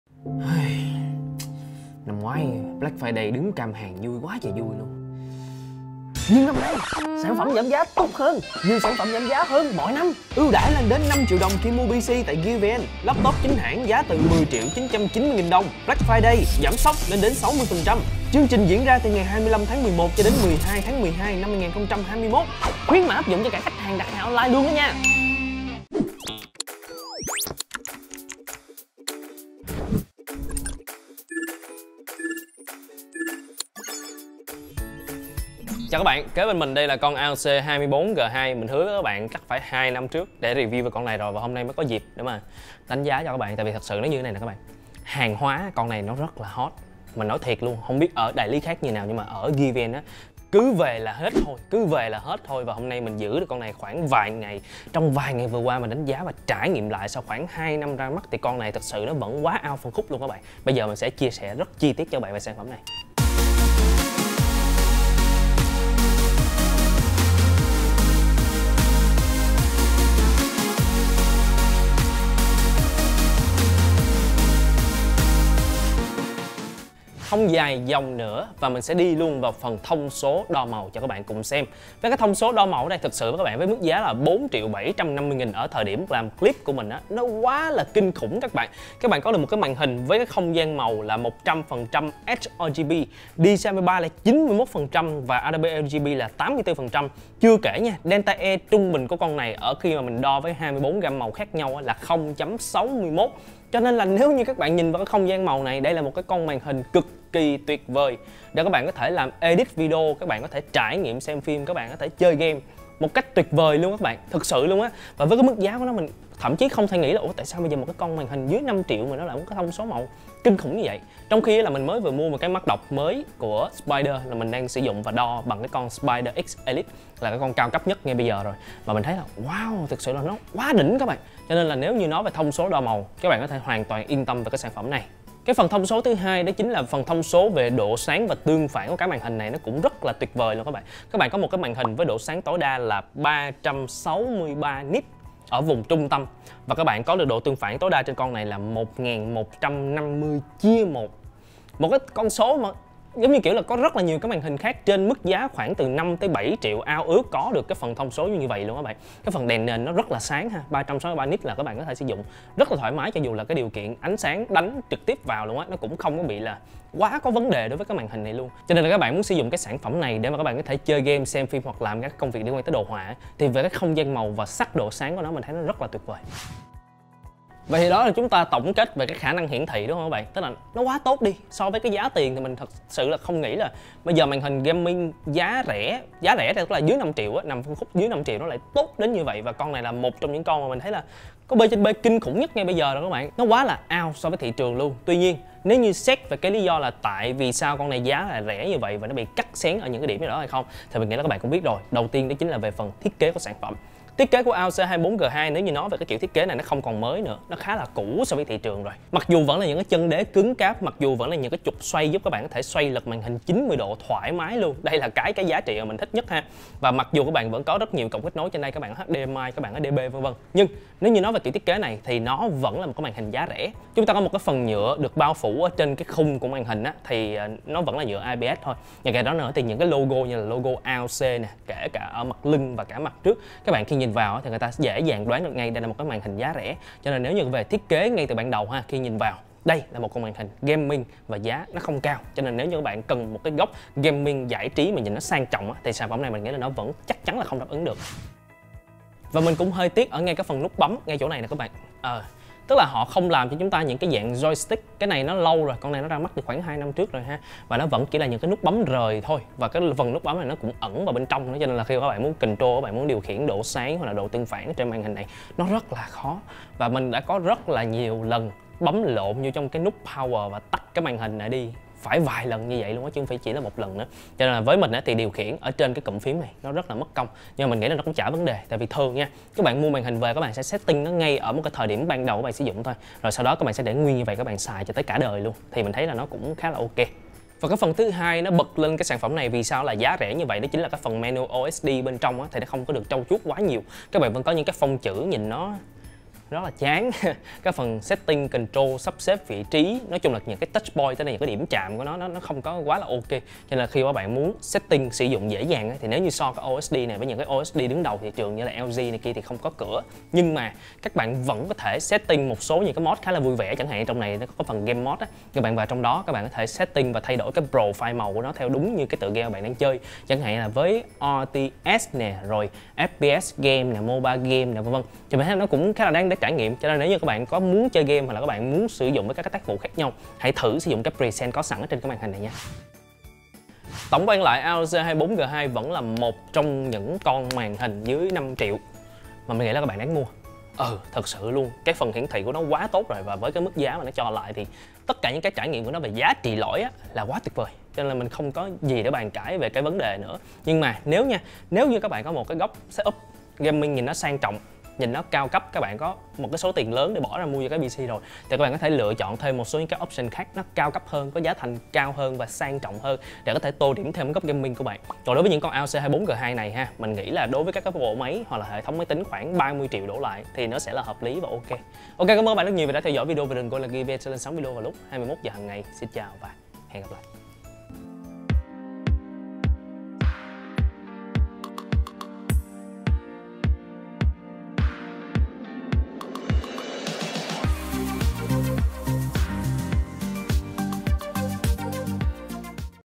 Đánh giá AOC 24G2 cuối năm 2021 | VẪN KHÔNG ĐỐI THỦ
0:00 Intro
0:36 Nói về sản phẩm này hiện tại
1:17 Về thông số đo màu
2:16 Về độ sáng và tương phản tối đa
2:57 Vì sao 24G2 lại rẻ thế?
4:09 Về nút bấm KHÓ THAO TÁC
5:33 Tổng kết lại

Hôm nay mình xin đánh giá chi tiết lại cho anh em mã AOC 24G2. Chắc chắn nói về mã màn hình này anh em không còn quá xa lạ nữa, vì ra mắt đã lâu. Tuy nhiên, còn ngon không? Mời anh em xem video
😍 Địa Chỉ:
Showroom HN: 37 NGÕ 121 THÁI HÀ, Q.ĐỐNG ĐA